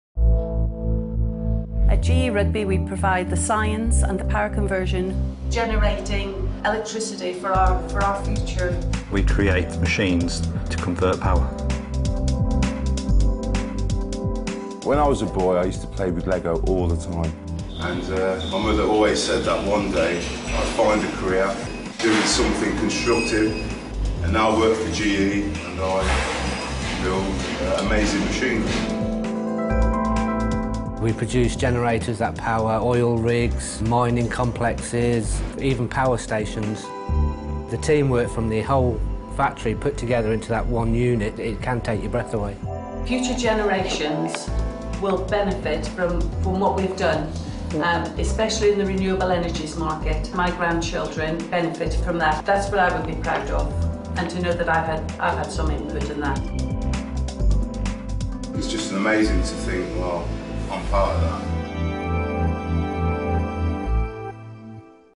Building the Economy - GE UK
Watch how the GE Energy team in Rubgy, UK are building the UK economy.

We're part of an 18,000 strong workforce, making up one of the largest manufacturing companies in the UK.  We're doing our bit for the UK.  ge.co.uk